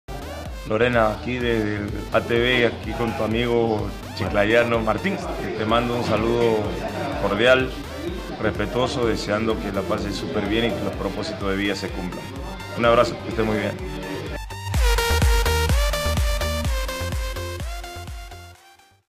Julio César Uribe - "El Diamante"
Julio César Uribe envía un saludo cordial para Lorena Martino.